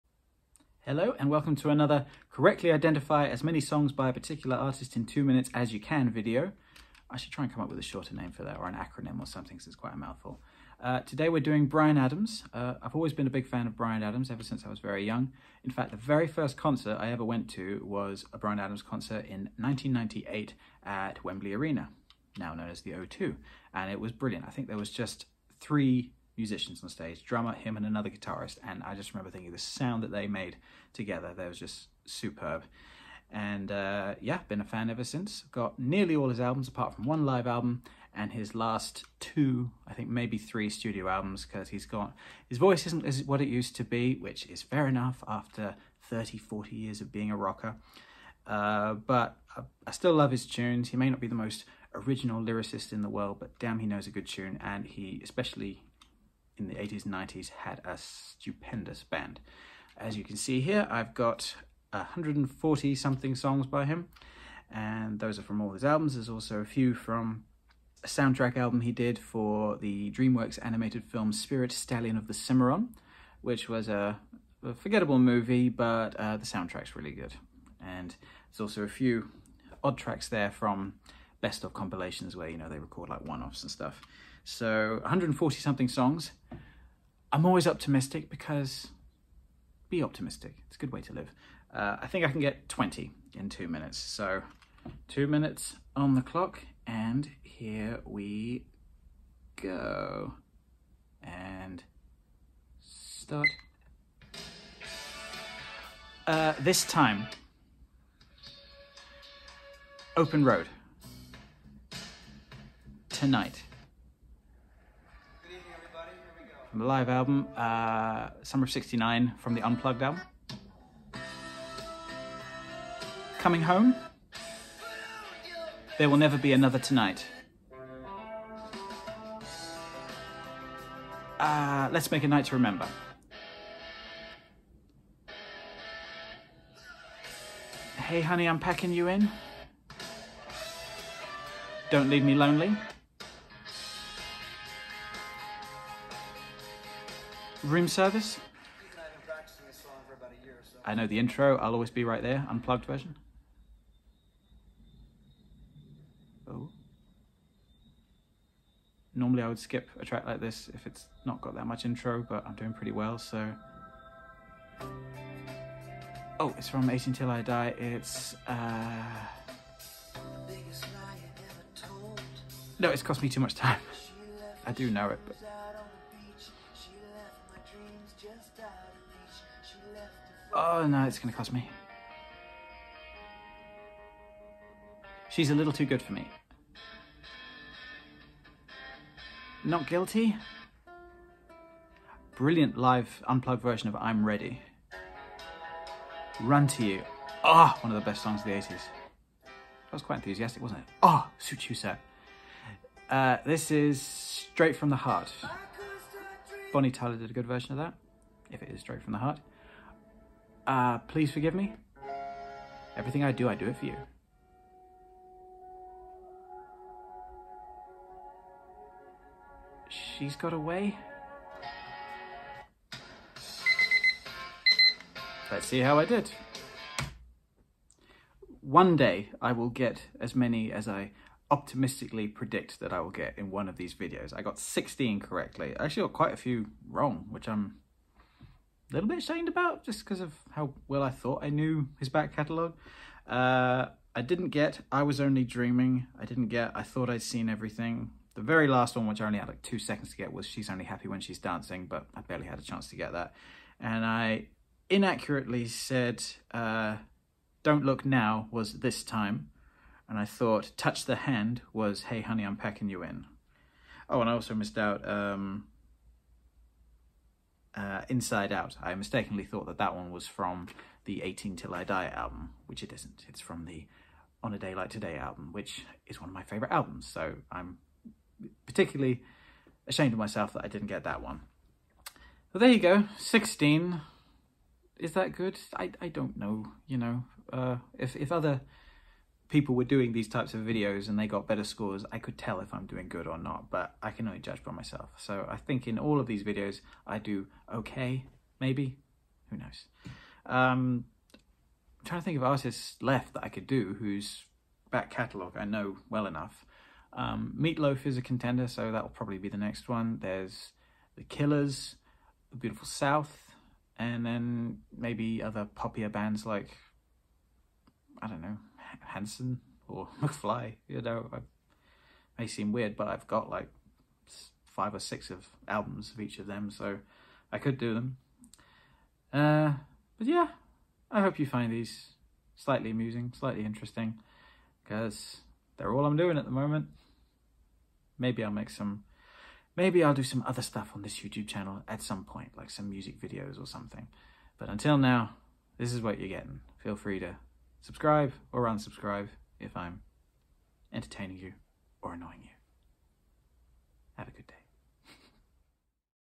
Identifying Bryan Adams songs - as many as I can in 2 minutes!
I consider myself a big Bryan Adams fan, so I decided to test my knowledge of his music by seeing how many of his songs I could correctly identify in 2 minutes from just hearing the opening few seconds.

I guessed 16 correctly, 2 incorrectly, and there were 3 I didn't know - not my best!